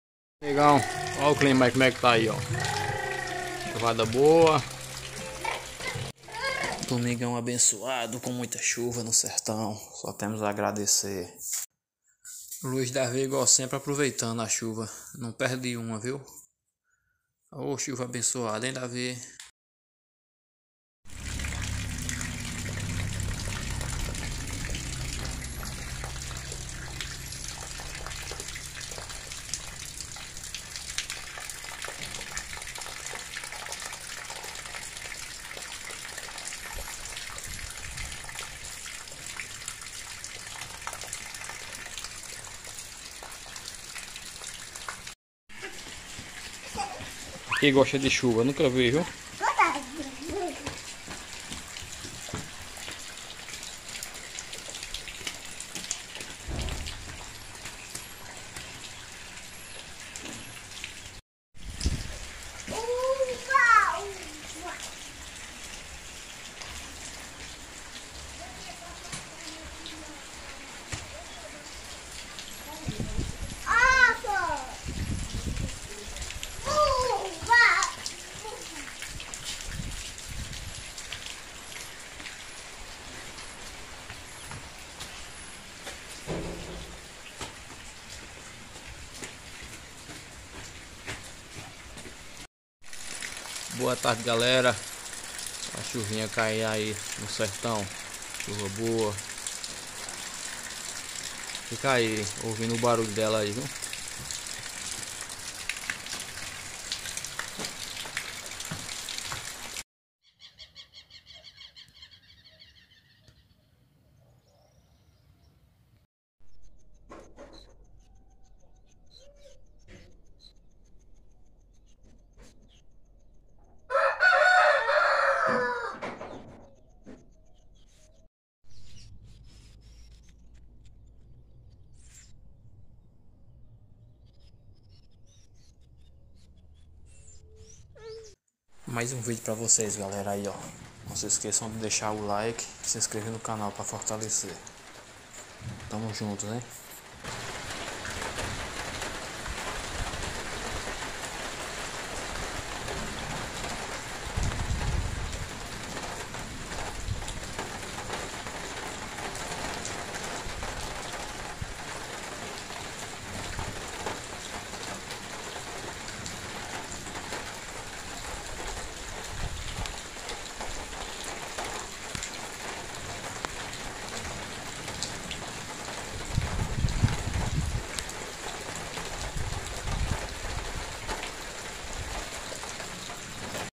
Chuva no Sertão Pernambuco
Tarde de chuva em nosso sertão, depois de um de um dia abafado.